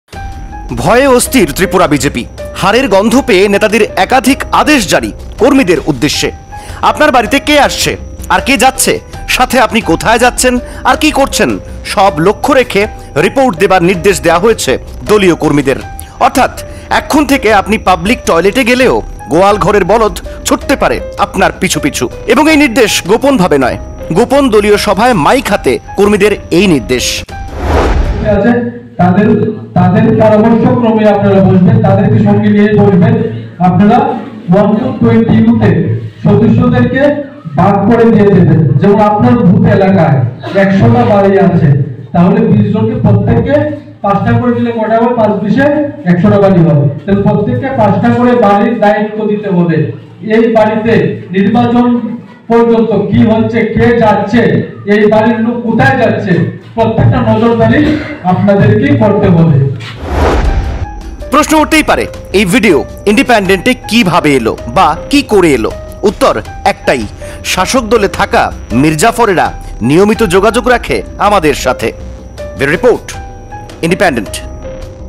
আপনার বাড়ীতে কে আসছে ? আপনি কোথায় যাচ্ছেন সব কিছুতে নজর রাখার নির্দেশ BJP কর্মীদের !ফাঁস গোপন ভিডিও
আপনি কি করছেন ? আপনার বাড়ীতে কে আসছে ? আপনি কোথায় যাচ্ছেন সব কিছুতে নজর রাখার নির্দেশ BJP কর্মীদের ! ফাঁস গোপন ভিডিও